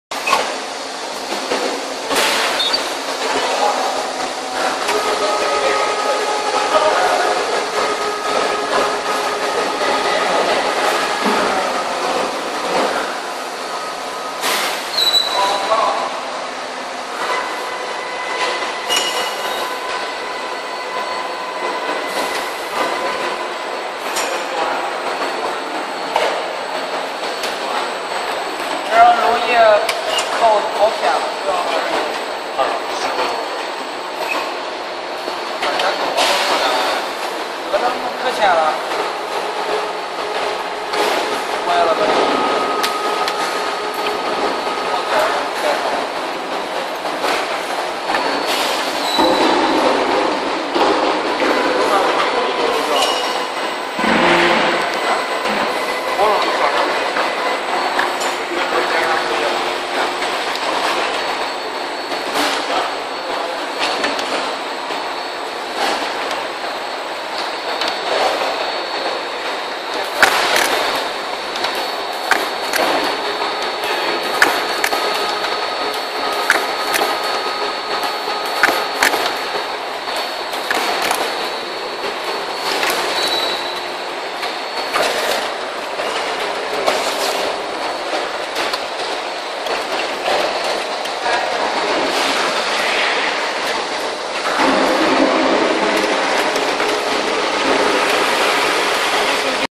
Scouring agent filling machine (MIC-ZF8 piston filling machine)
This piston filling machine is suitable for:
Lube oil, engine car oil, industry oil, olive oil, bean oil, paint, and other similar products.

This machine function is:
1.      Automatic bottle sensing system and filling
2.      No bottle no filling
3.      Filling error is control within 1‰
4.      Filling arrange : mini volume: max volume =1:10
5.      Automatic filling volume correction system.
6.      Can be customized for different bottle and caps.
7.      It can be equipped with a cleaning system.